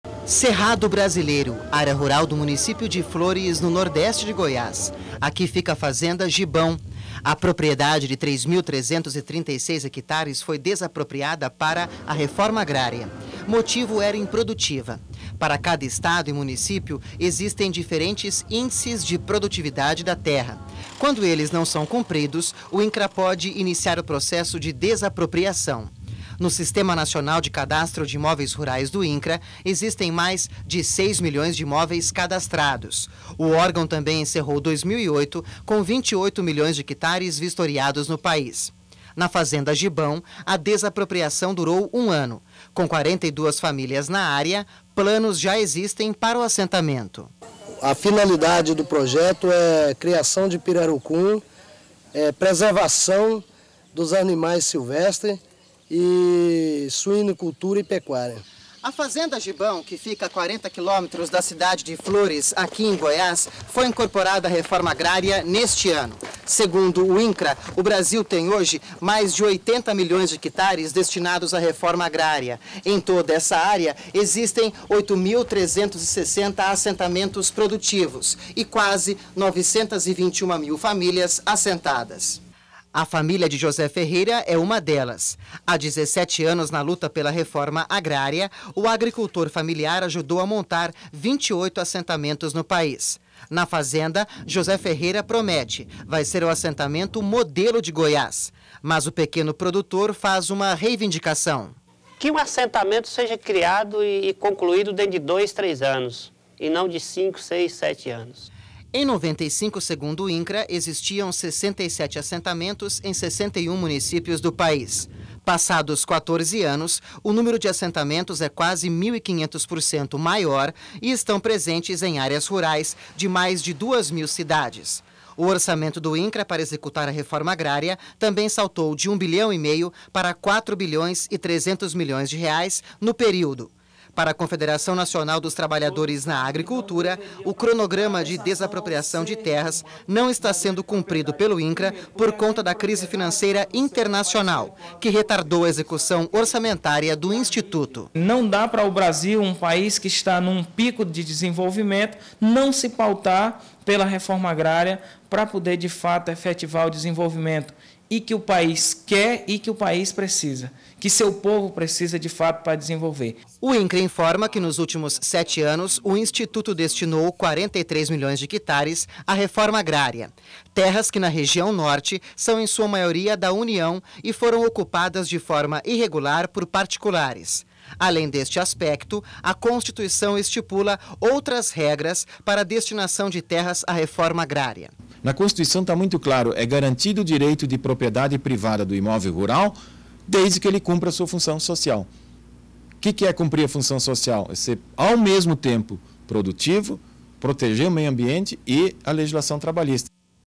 Reforma Agrária - Balanço do Incra
Nos últimos sete anos, o Incra destinou 43 milhões de hectares de terra à reforma agrária. E o orçamento saltou de UM bilhão e MEIO para mais de QUATRO bilhões de reais. Veja o balanço do INCRA nesta reportagem especial.